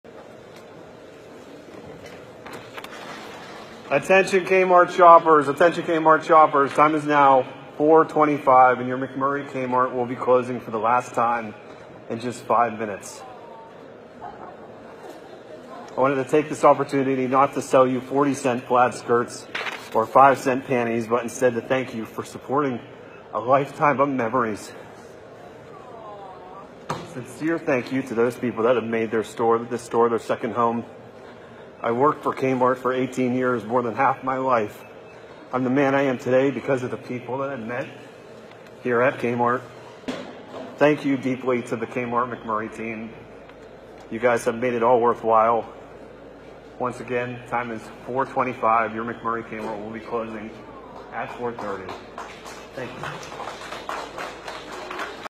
Kmart manager delivers an emotional goodbye, closing his store one final time
"Thank you for supporting a lifetime of memories."

In the wake of Sears filing for Chapter 11 bankruptcy, a Pennsylvania Kmart manager delivers an emotional goodbye, closing his store one final time.